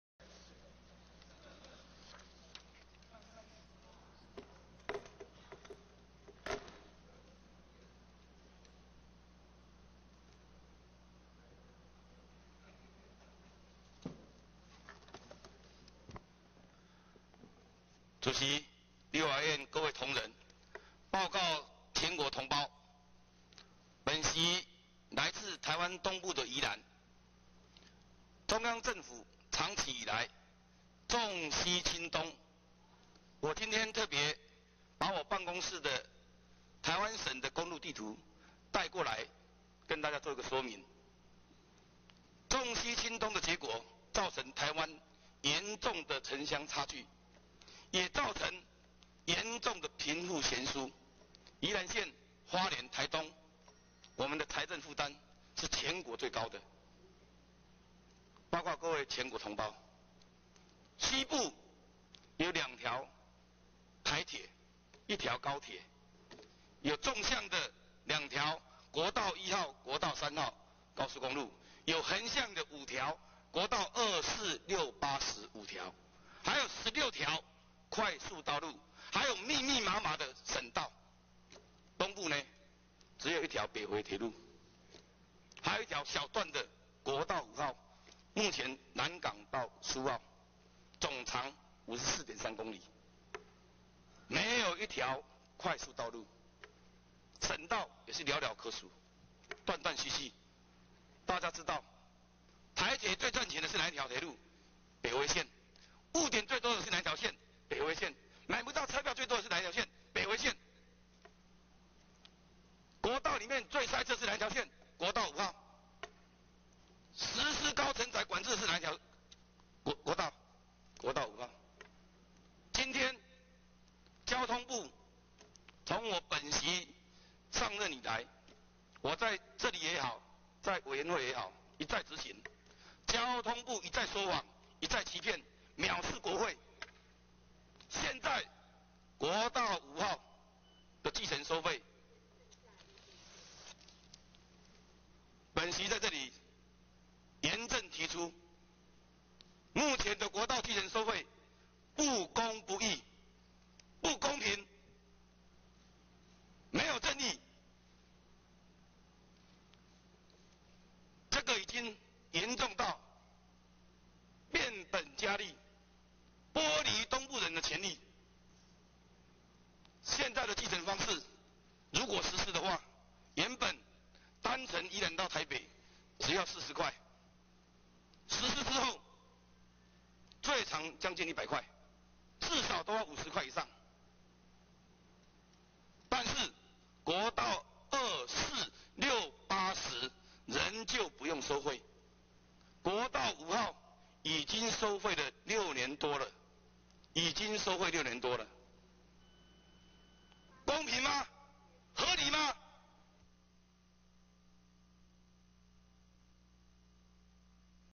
2013-05-03 陳歐珀 發言片段, 第8屆第3會期第11次會議
會議簡介: 第8屆第3會期第11次會議一、3日上午9時至10時為國是論壇時間。二、討論事項：本院委員李慶華等33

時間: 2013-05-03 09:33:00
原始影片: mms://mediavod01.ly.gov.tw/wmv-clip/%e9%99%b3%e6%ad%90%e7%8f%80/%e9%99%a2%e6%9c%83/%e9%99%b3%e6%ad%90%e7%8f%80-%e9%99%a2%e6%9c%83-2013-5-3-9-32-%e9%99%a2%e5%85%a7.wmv